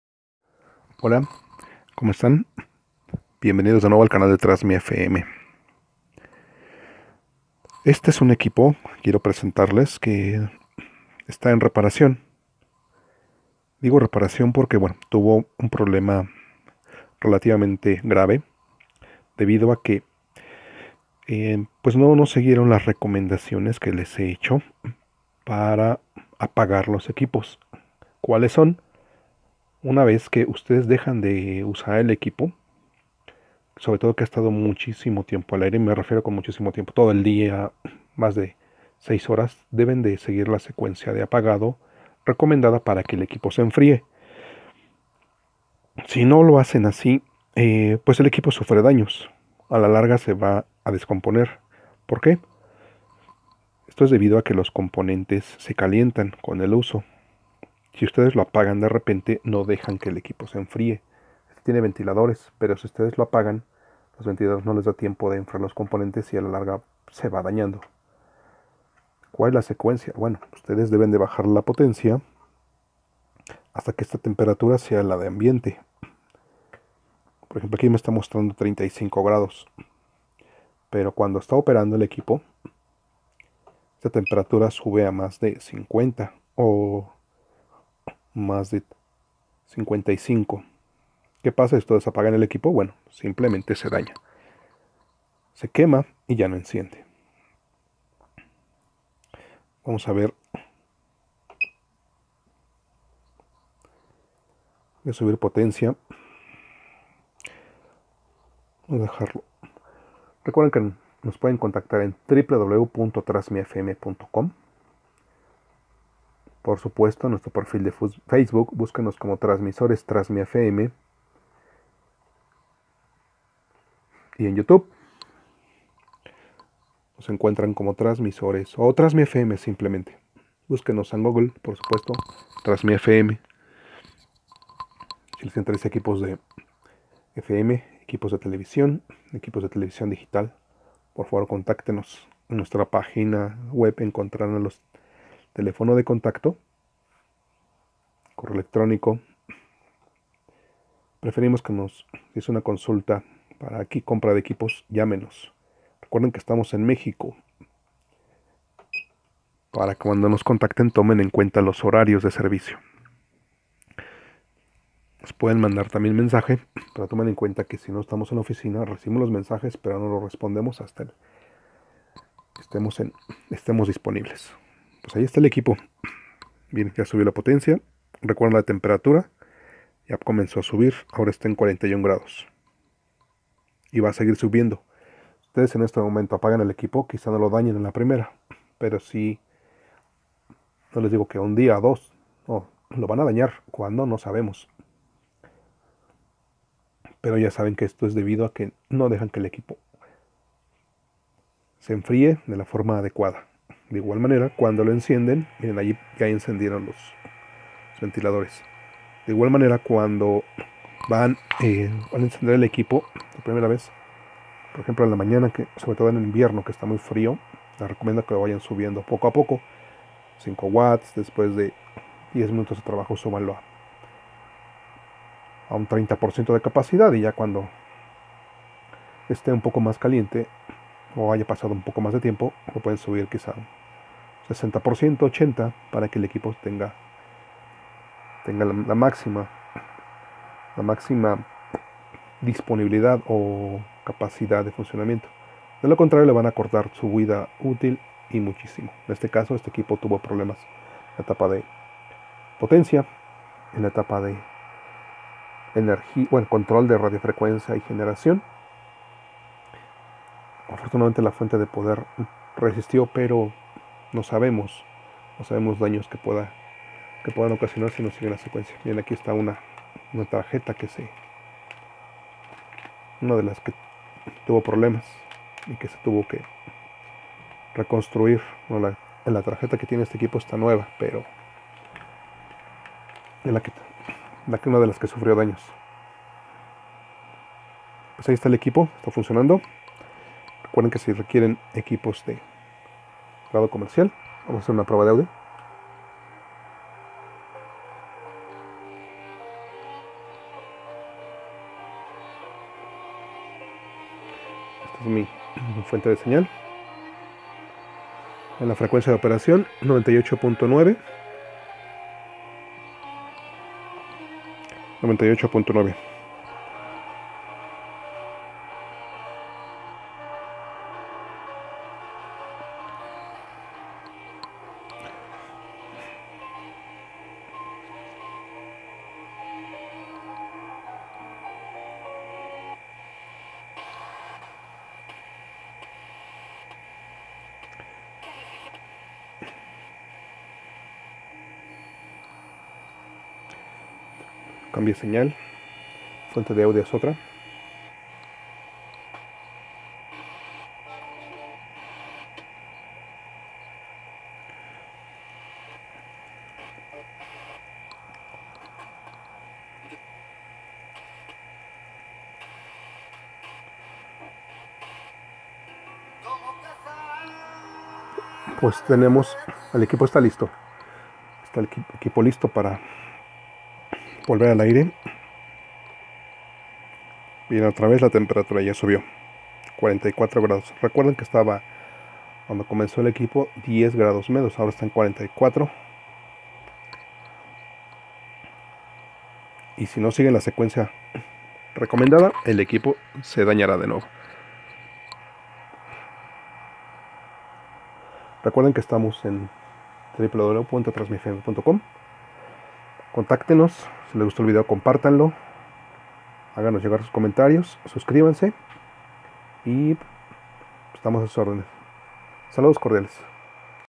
Transmisor FM 50 watts envío Querétaro, México.
Pruebas finales en laboratorio TRANSMIFM a Transmisor FM 50 watts stereo que sufrió daños ocasionados por no seguir la secuencia de apagado recomendada. Ya esta listo para salir al aire de nuevo!